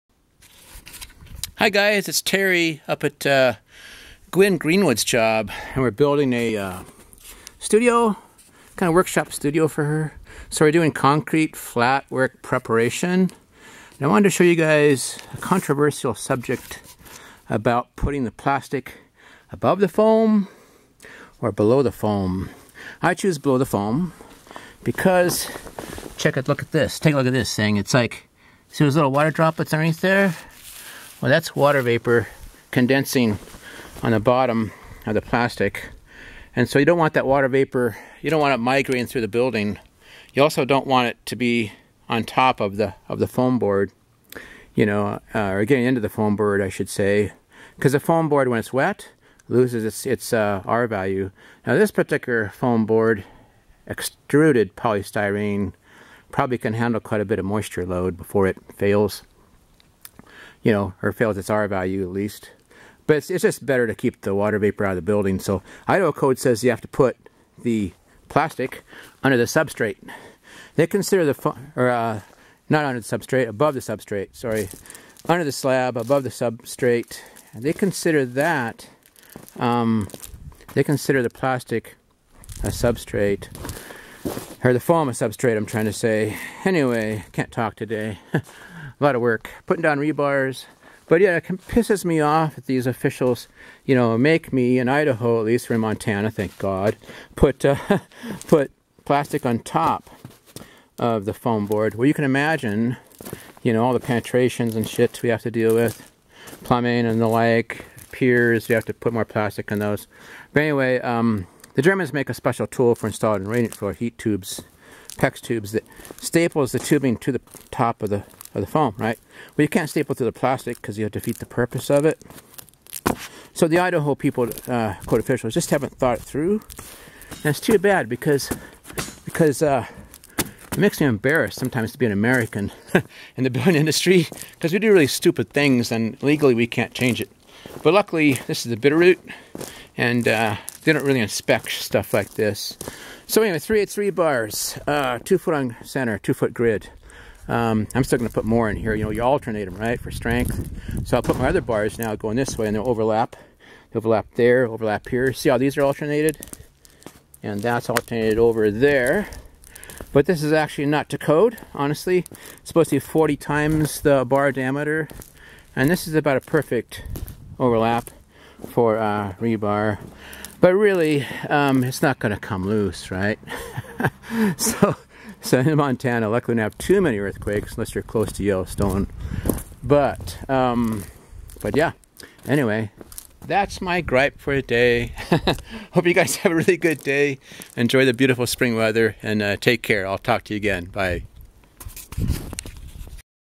Under slab plastic below foam insulation.
Plastic should be installed first, under the foam insulation, then pour the concrete slab.  Keep the water vapor out of the building.  And the foam board is part of the building, despite some State's wrong building code ideas of placing the plastic above the foam, causing it to absorb water vapor, loosing insulation R value and possibly growing mold.